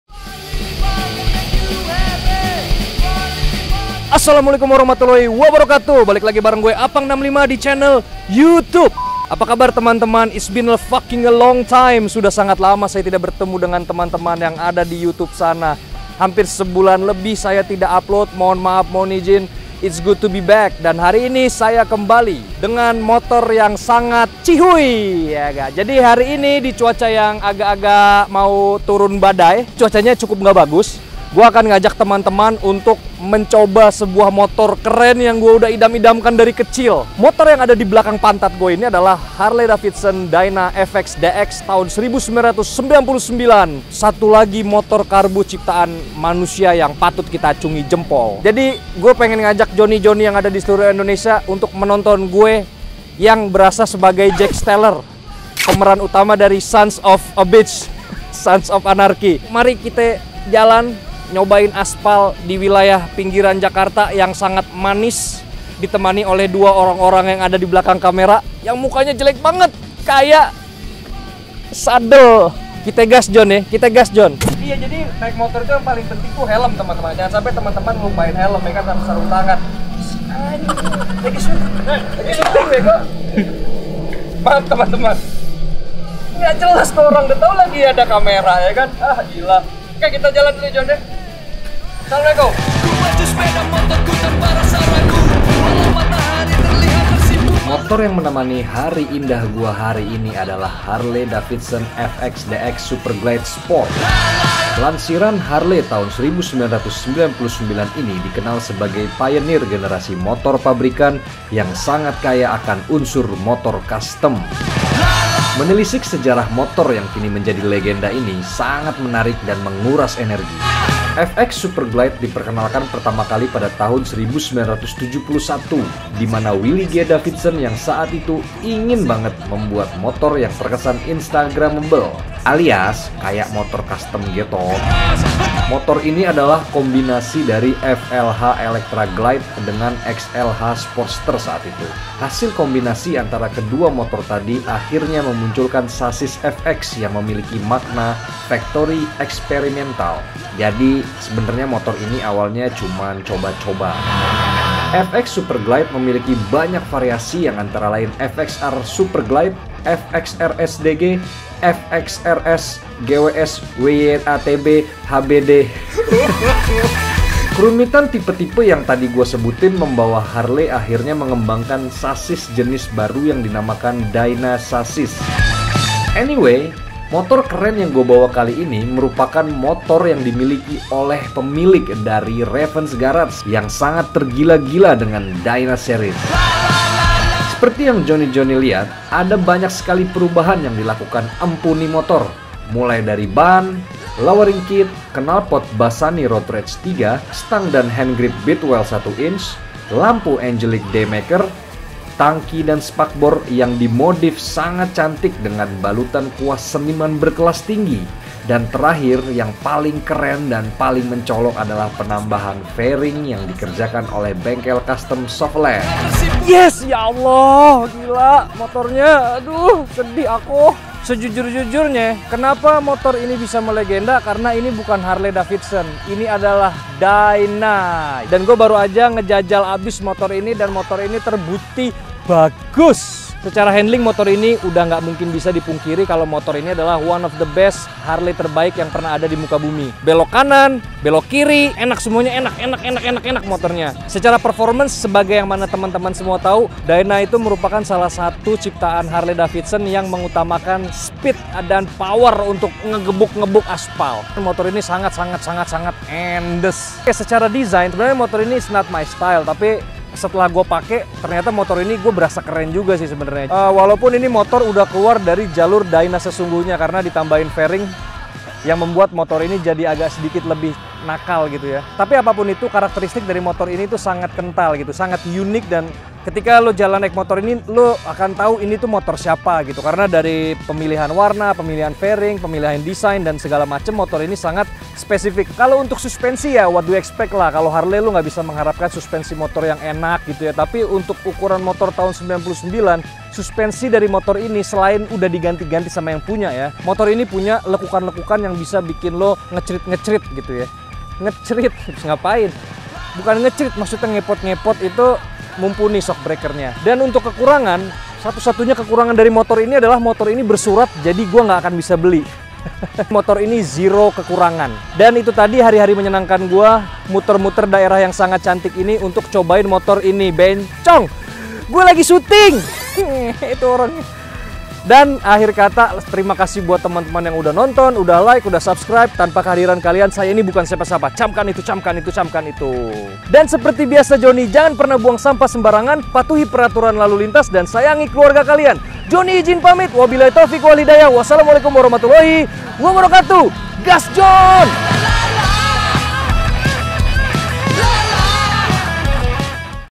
Harley Davidson FXDX ganteng Apunk65 Eps.42
Jajal salah satu motor yang sudah gua impikan sejak kecill!!! bukan Harley, tapi ini Dyna!! kebetulan sang owner tergila gila sama Dyna! mau tau kisah keren motor ini? yuk tonton jonnn!!

Samlekum.